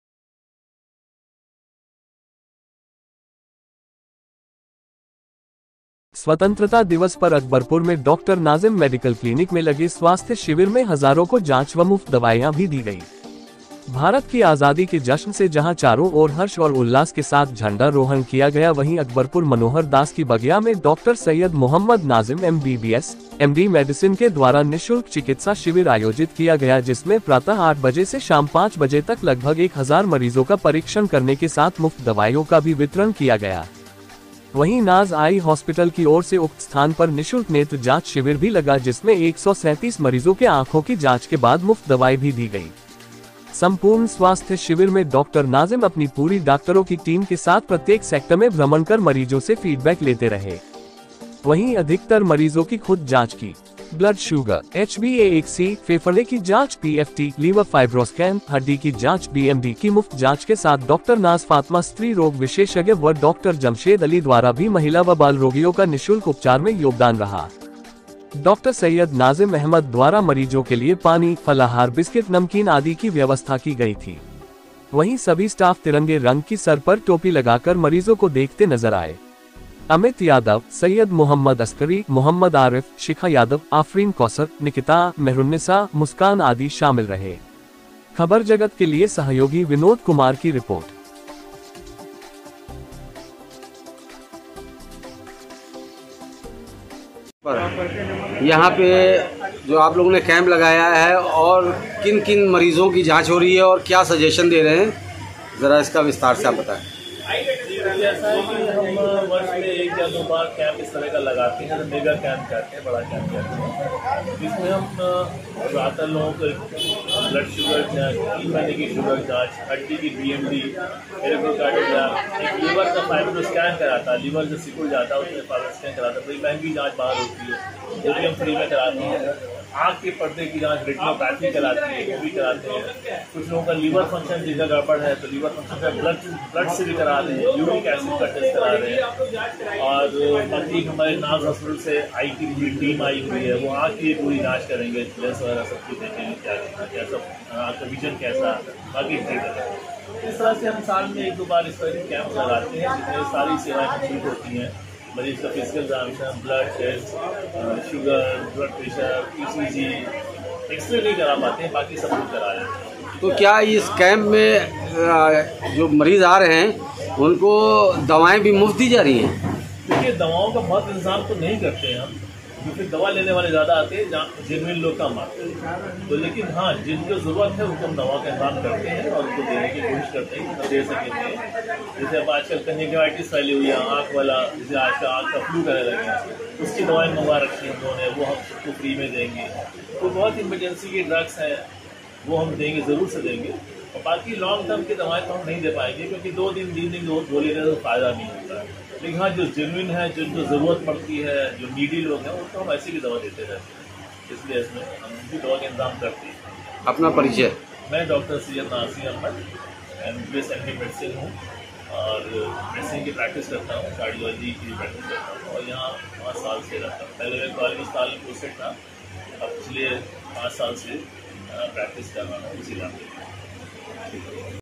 स्वतंत्रता दिवस पर अकबरपुर में डॉ नाज़िम मेडिकल क्लीनिक में लगे स्वास्थ्य शिविर में हज़ारों को जांच
स्वतंत्रता दिवस पर अकबरपुर में डॉ नाज़िम मेडिकल क्लीनिक में लगे स्वास्थ्य शिविर में हज़ारों को जांच व मुफ्त दवाईयां भी दी गई